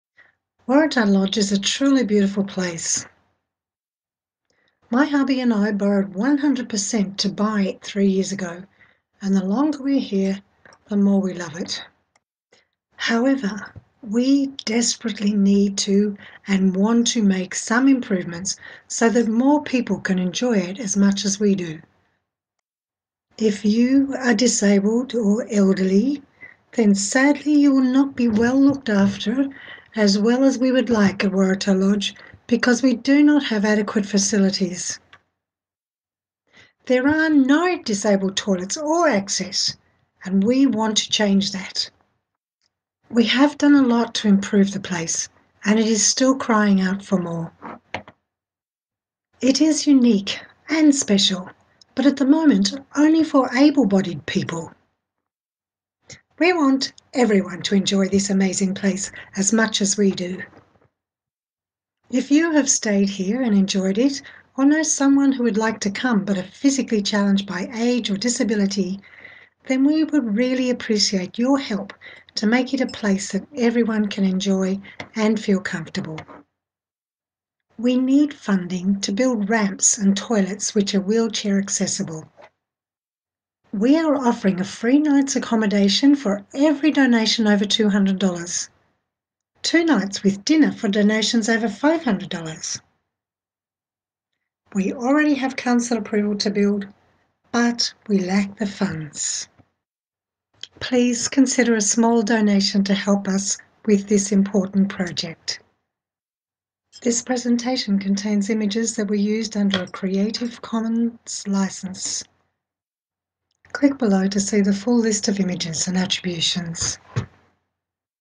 video disabled facilities
We need a new loo or two  at Waratah Lodge, a ramp as well, then we can look after everyone